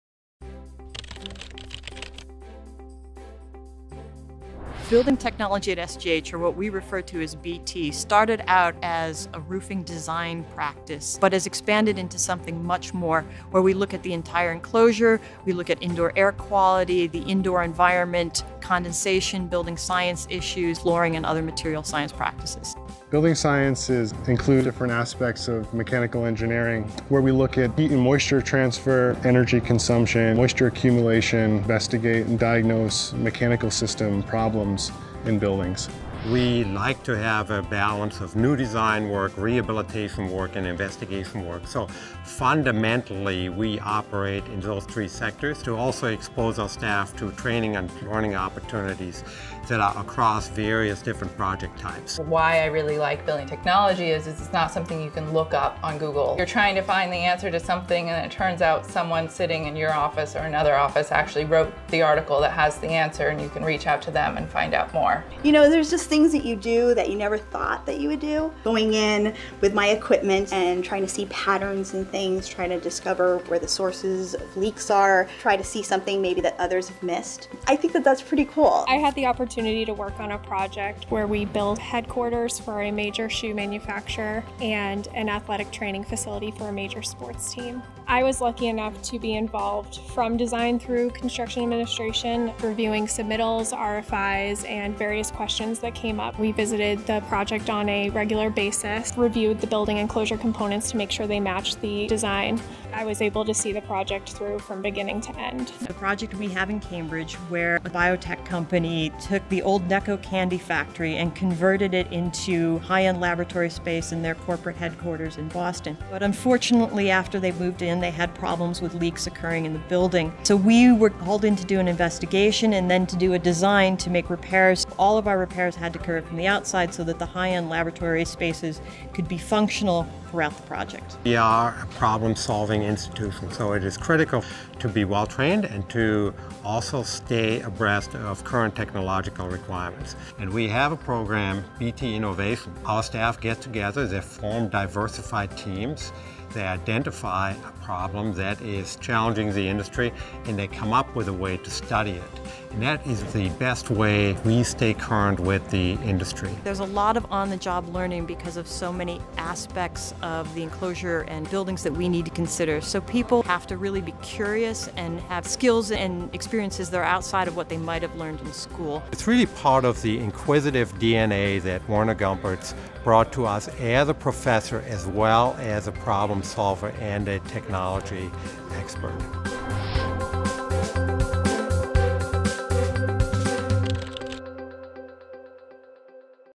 What Is Building Technology?
Take a look to learn about Building Technology and what it takes to be a part of the BT team at SGH.